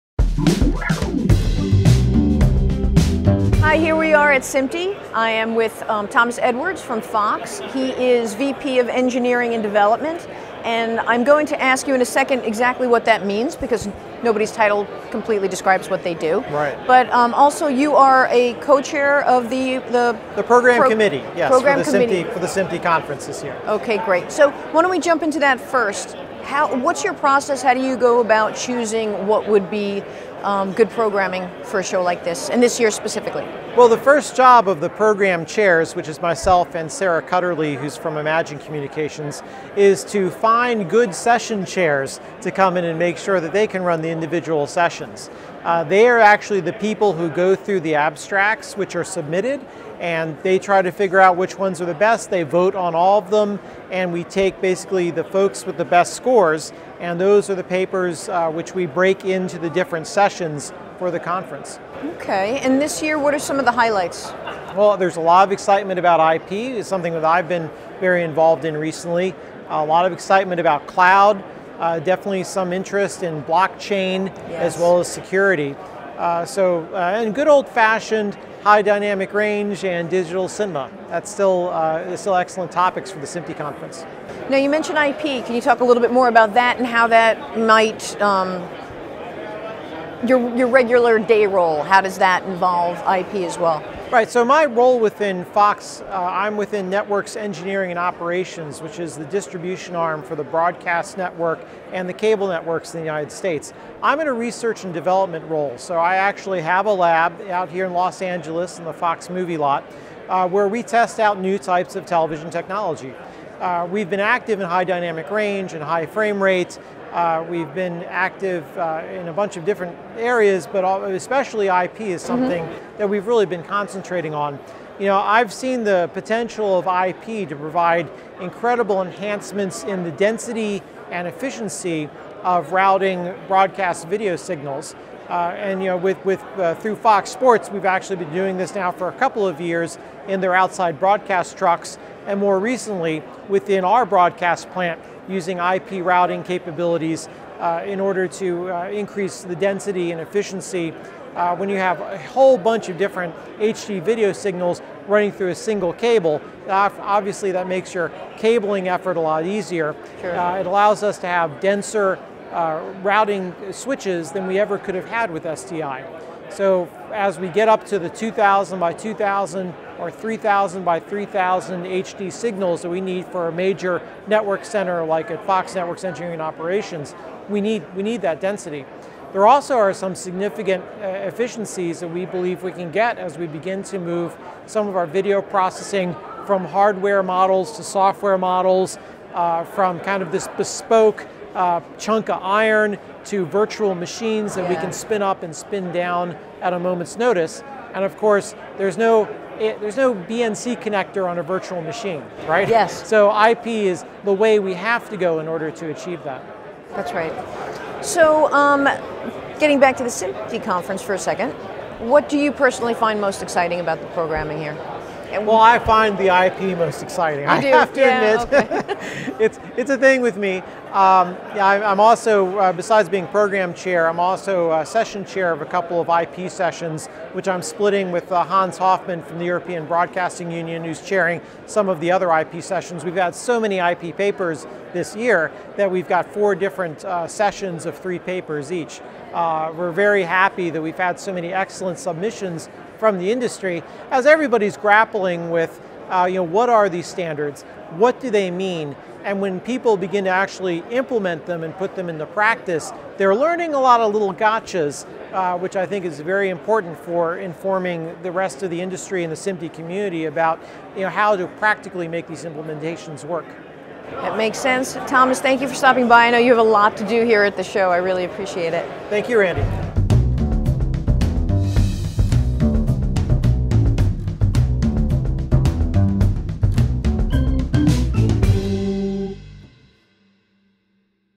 SMPTE 2018: Fox VP of engineering and development Thomas Edwards
Thomas Edwards was the co-chair of SMPTE 2018's program committee. He talks about the process of choosing which submitted papers become sessions. He also talks about his role at Fox.